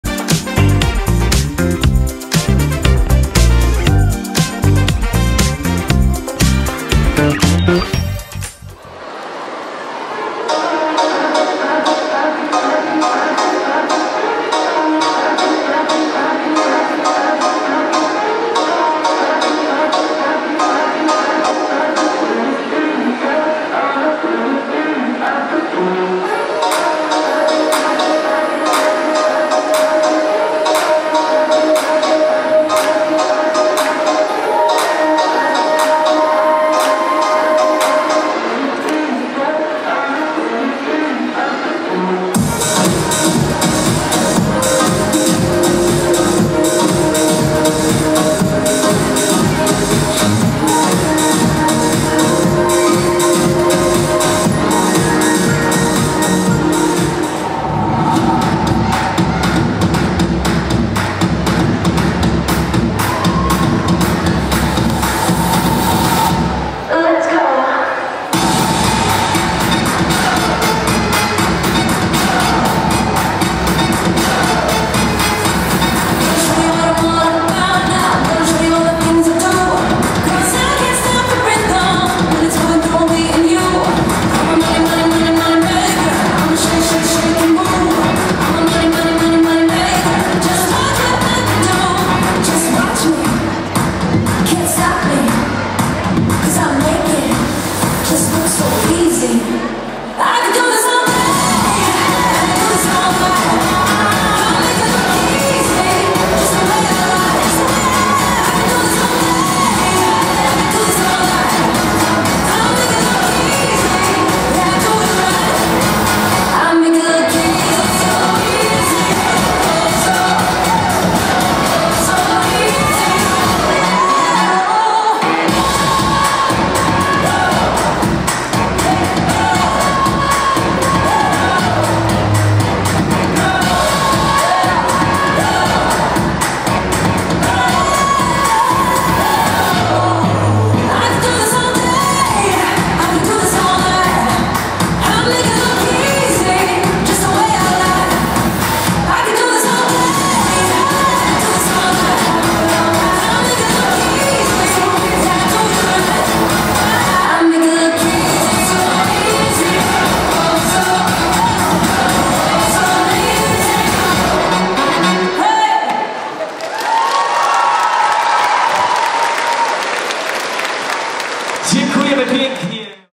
#8 6-7-Latki - Grupa Karoliny Moryń [Fame Dance Festival 12]
Występ grupy 6-7 latek podczas gali finałowej Fame Dance Festival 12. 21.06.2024 w Płockim Amfiteatrze. Trenerką grupy jest Karolina Moryń

Zajrzyjcie na nasze media społecznościowe!

Nagranie i montaż - Pineapple Media
Miniatura - Małgorzata Kułakowska
Zdjęcie na miniaturze - Paweł Jakubowski